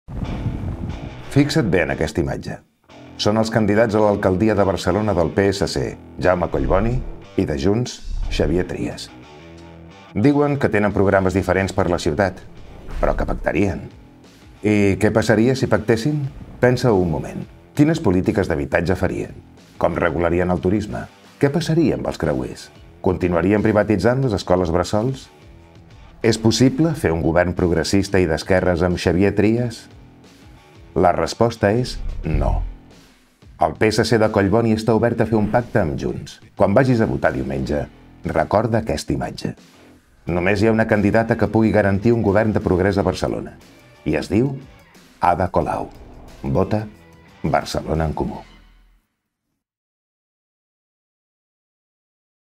Govern progressista amb Trias?
Ada Colau és l'única que garanteix un govern progressista. Recorda que si votes Collboni, pots fer alcalde a Trias.